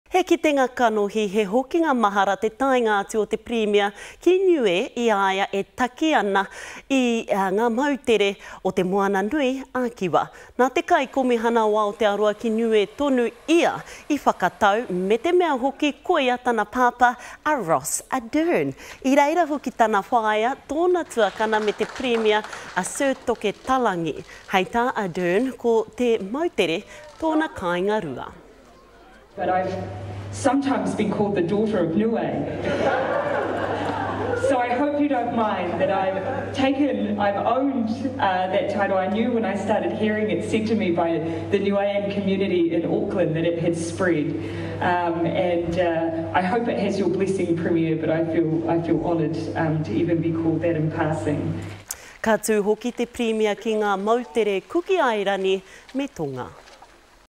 PM arrives in Niue to warm whānau welcome
It was a family affair for the Prime Minister as she arrived in Niue on her week-long tour of the Pacific Islands today. She was greeted by the NZ High Commissioner to Niue, her father Ross Ardern. Along with Premier Sir Toke Talagi her mother, sister and niece were also part of the welcoming party.